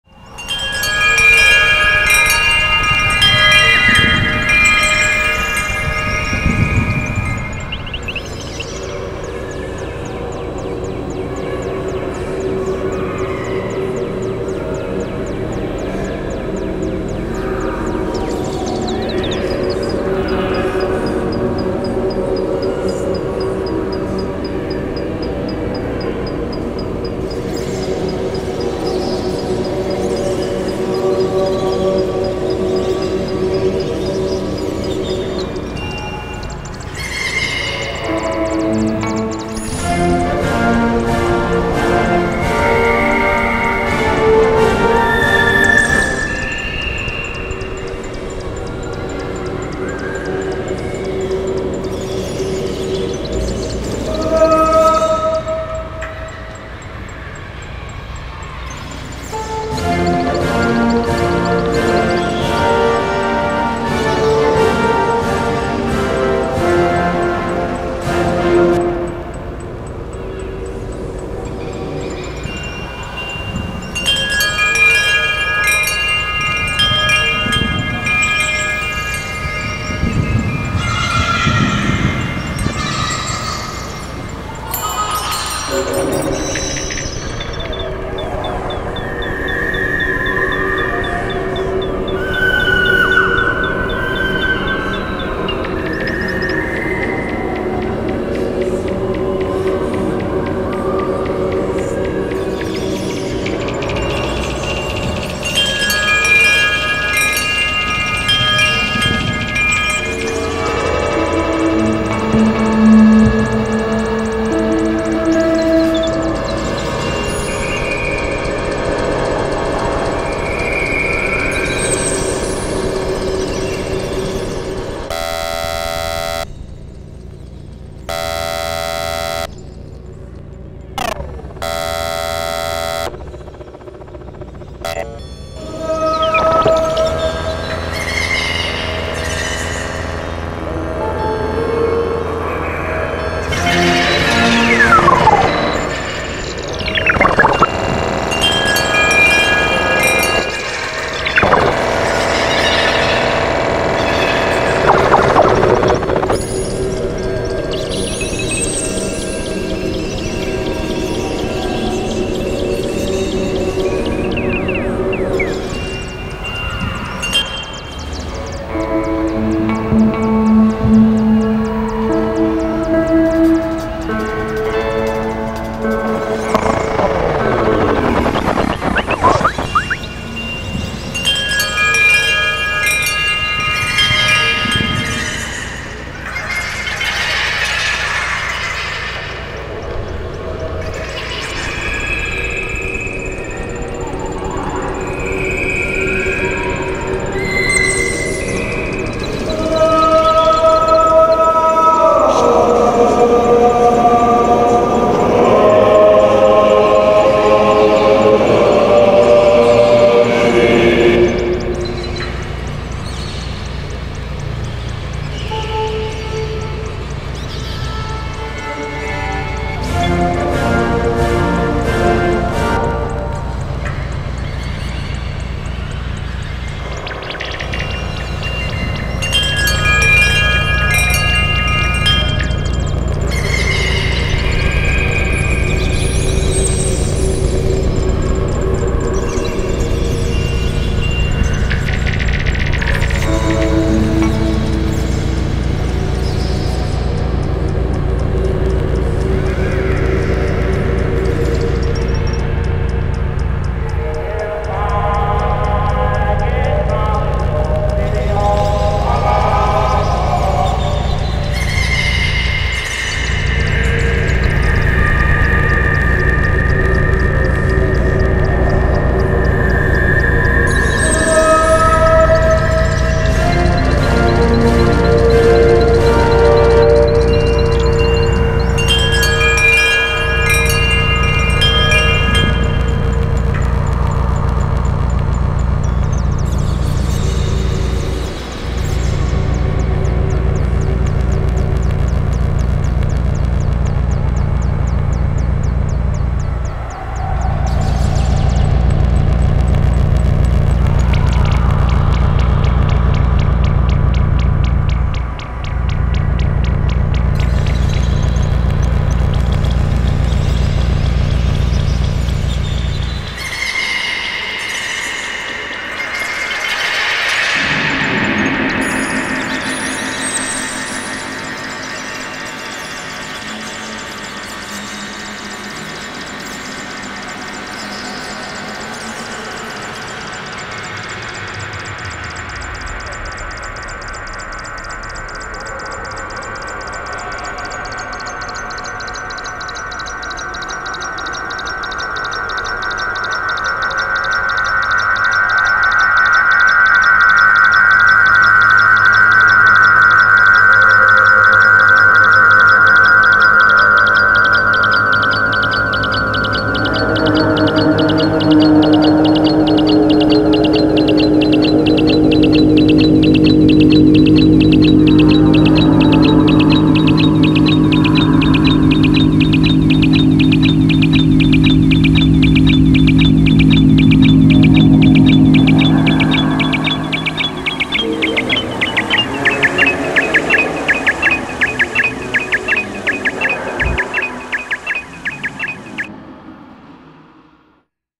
10 29 21 Buchla improvisation [Part 2]
There are 2 x KESAKO Player & Reverb in the system as sound sources and reverb, as well as a Scrotum Lab "Espace temps" (an adaptation of a Boss DD-5 digital delay pedal + Wooglebug in one module).

No external effects added.